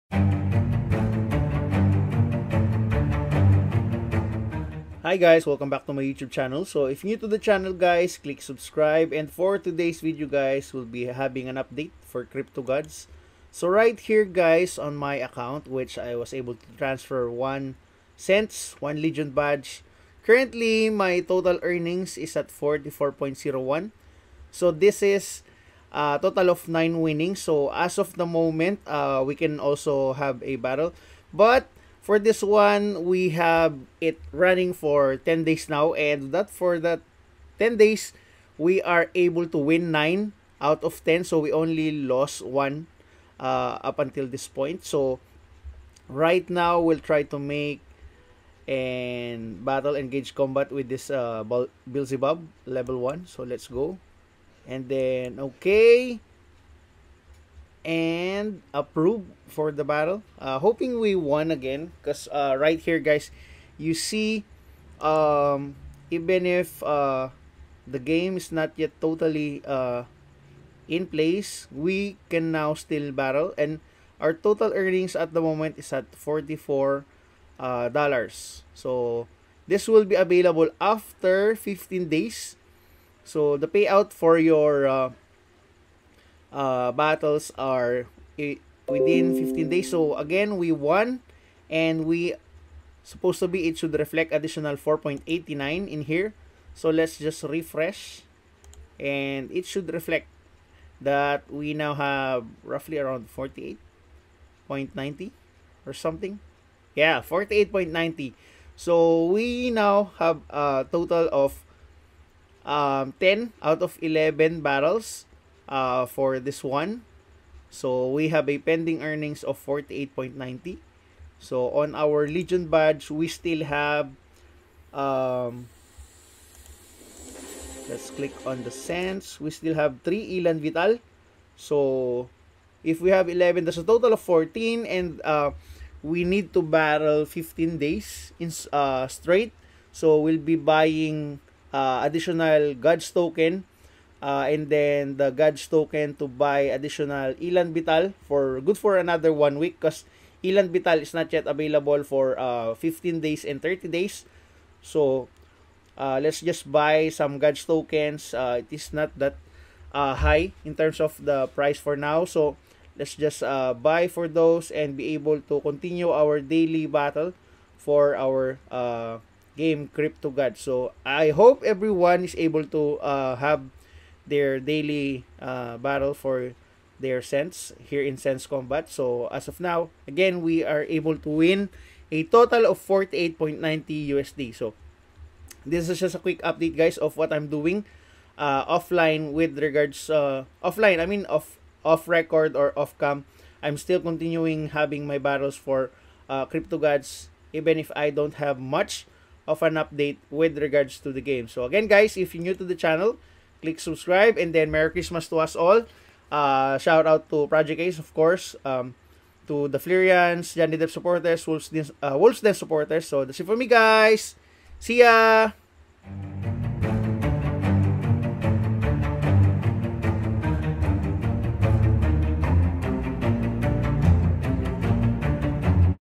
The Advanced Guide To CryptoGodz: Earnings Update
In today's video, I will make an update on my earnings for my one Sentz and one Badge account for CryptoGodz.

ultm8 x gaming | #5151